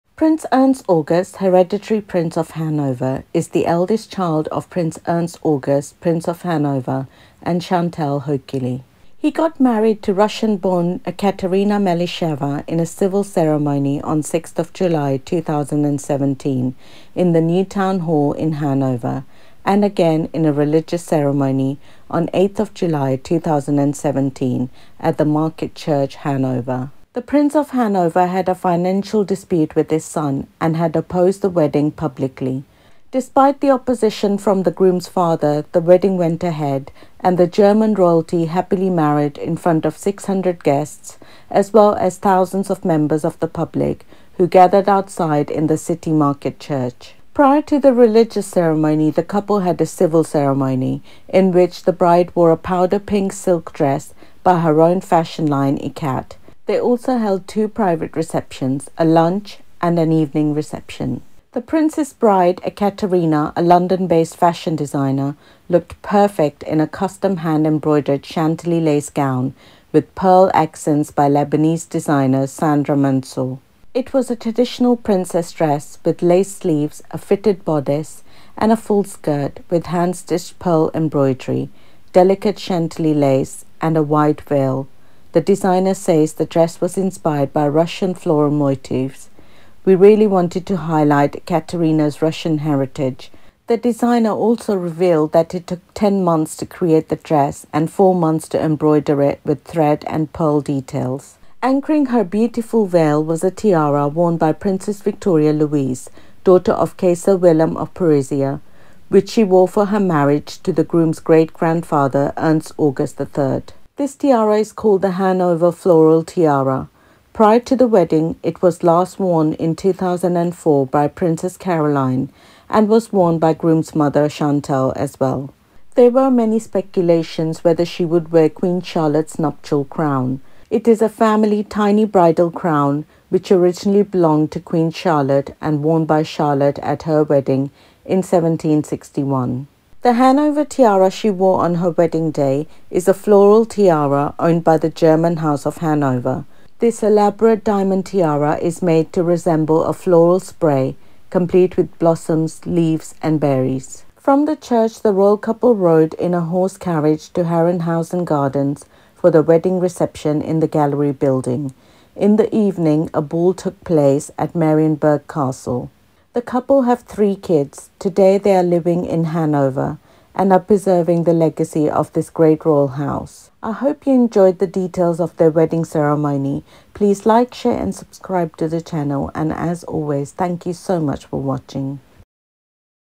Prince Ernst August And Princess Ekaterina Of Hanover’s Royal Wedding
Prince Ernst August, Hereditary Prince of Hanover is the eldest child of Prince Ernst August, Prince of Hanover and Chantal Hochuli